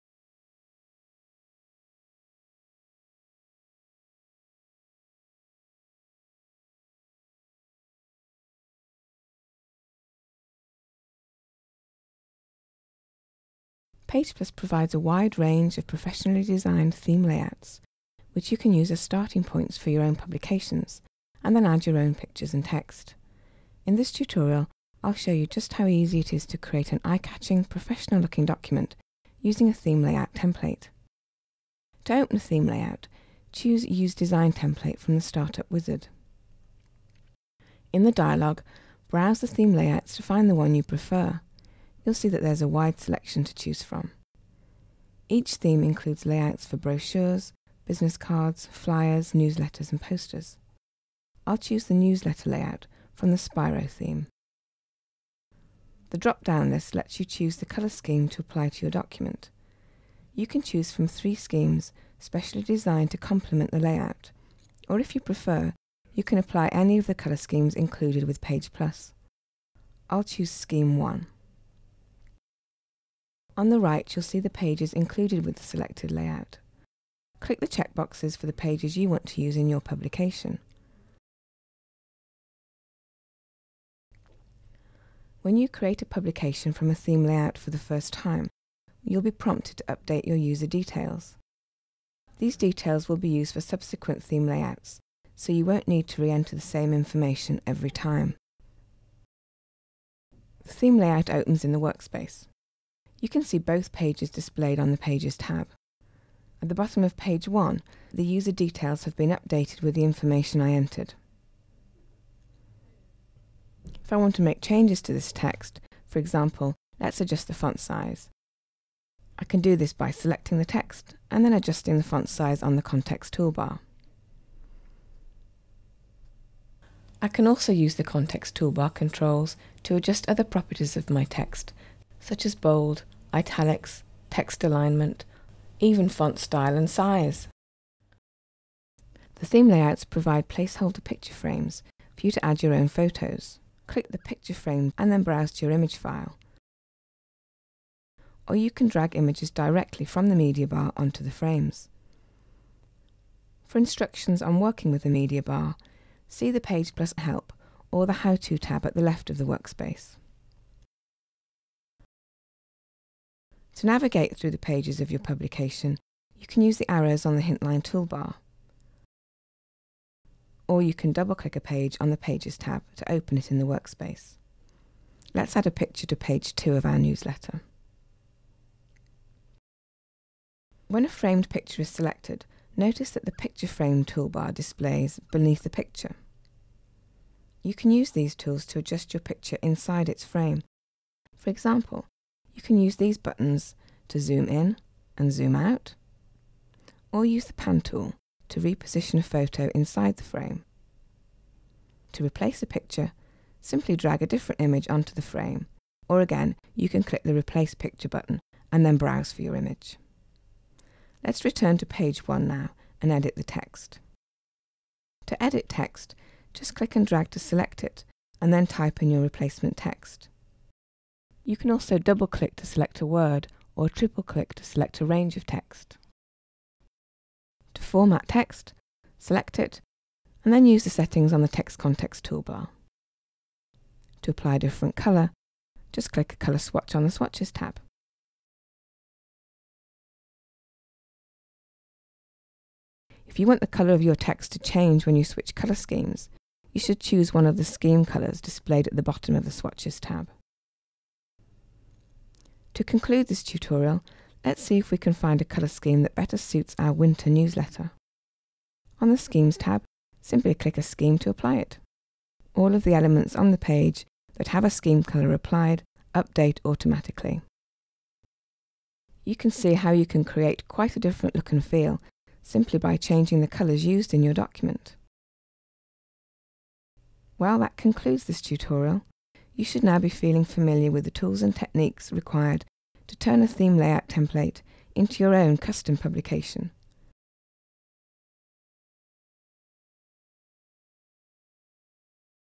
Serif PagePlus X5 Tutorial - Quick Start
Get to grips with Serif PagePlus X5 in the quickest way possible -- with a guided introduction to the workspace from a PagePlus expert!